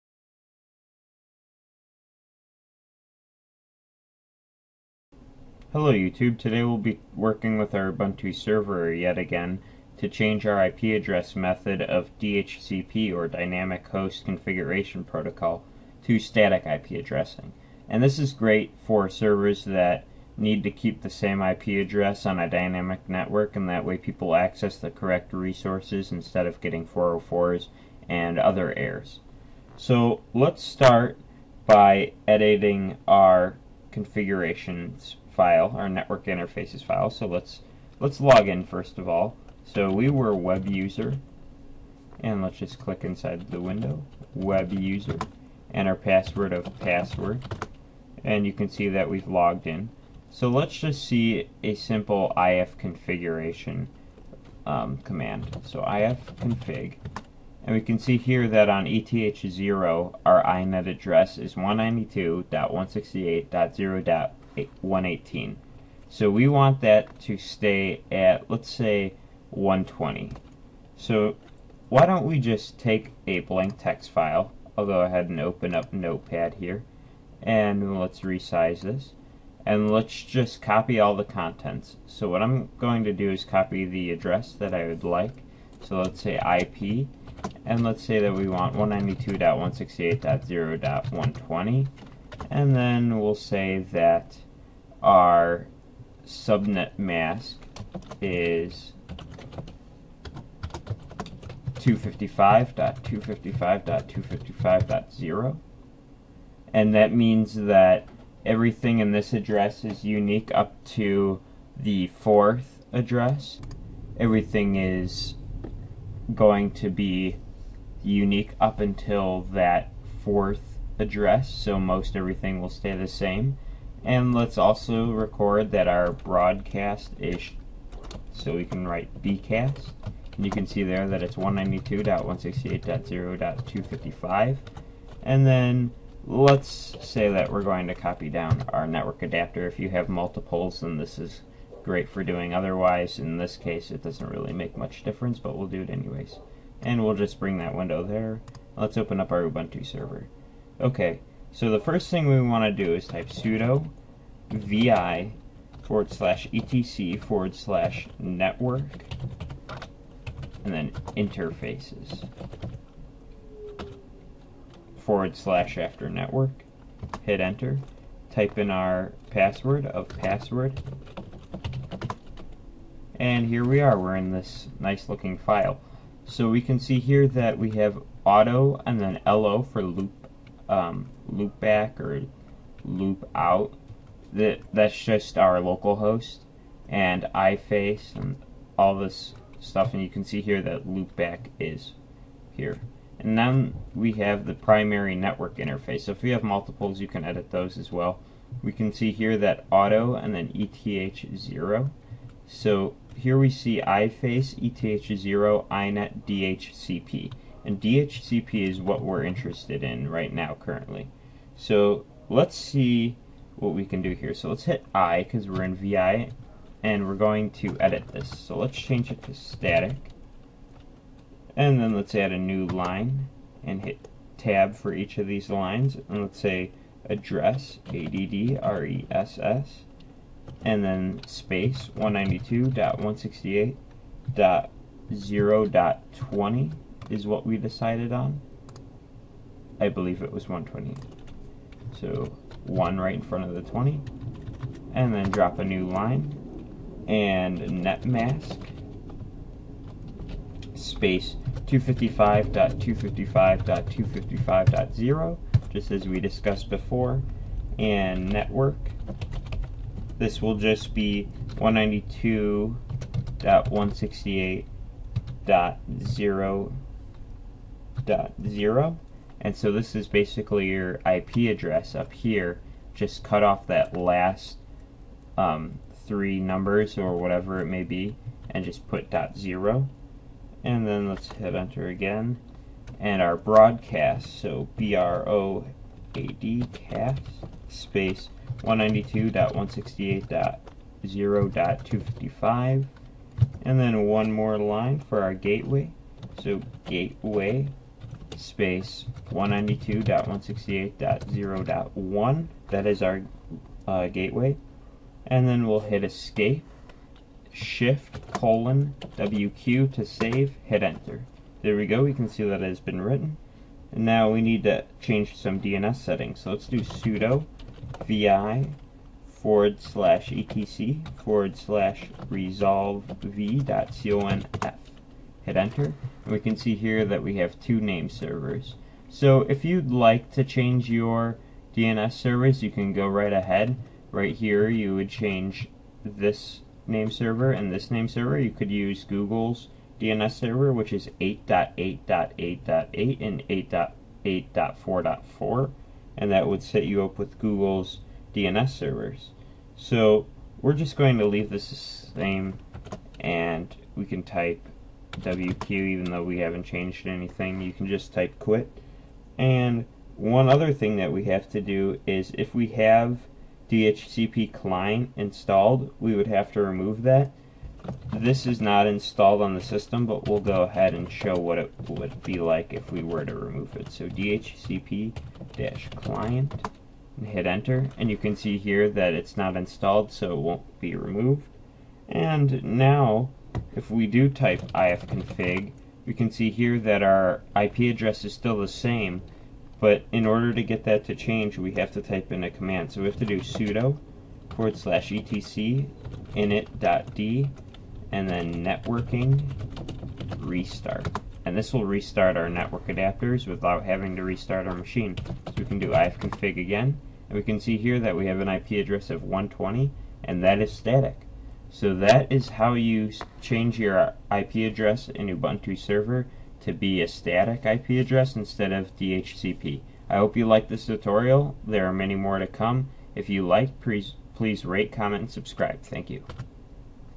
[Linux] Tutorial: Setting a static IP in Ubuntu Server [HD]
This video will demonstrate setting up a static IP in Ubuntu Server.

Commands used in this tutorial(seperated by one blank line):

ifconfig

sudo vi /etc/network/interfaces

sudo vi /etc/resolv.conf

sudo apt-get remove dhcp-client

sudo /etc/init.d/networking restart